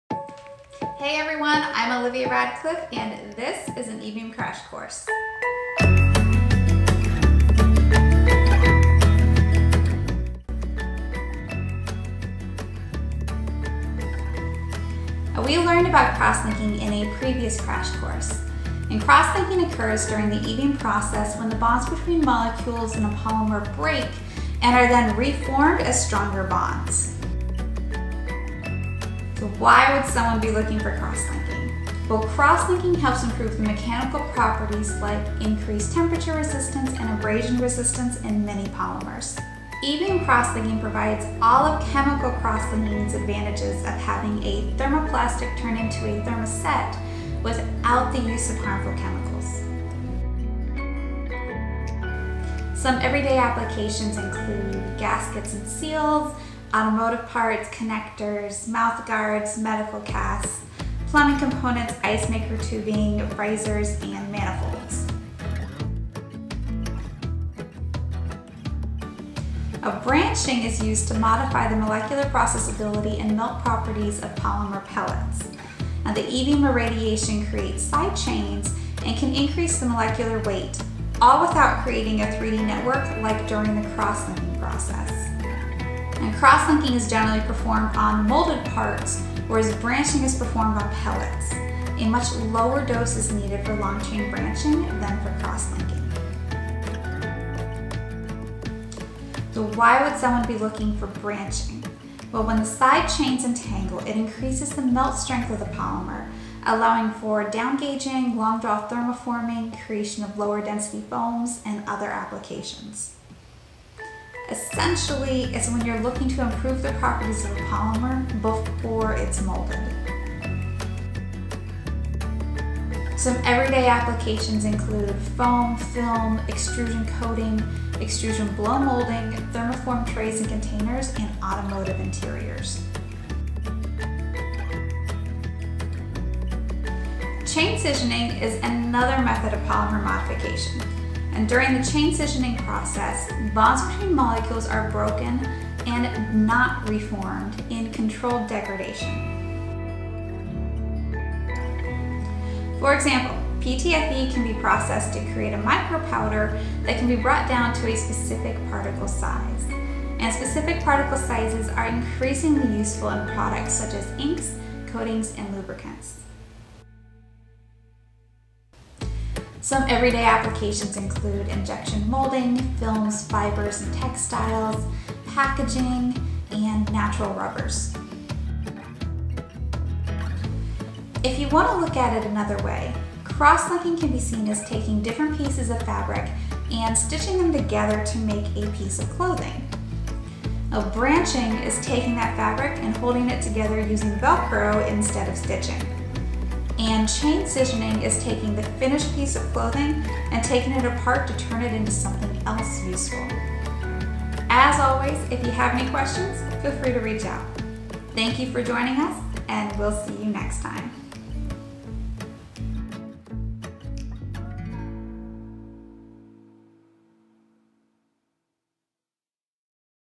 E-BEAM Crash Course: 3 Types of Polymer Modification (Beginner Level)
Join the E-BEAM team to learn about 3 types of polymer modification: crosslinking, branching, and chain scissioning.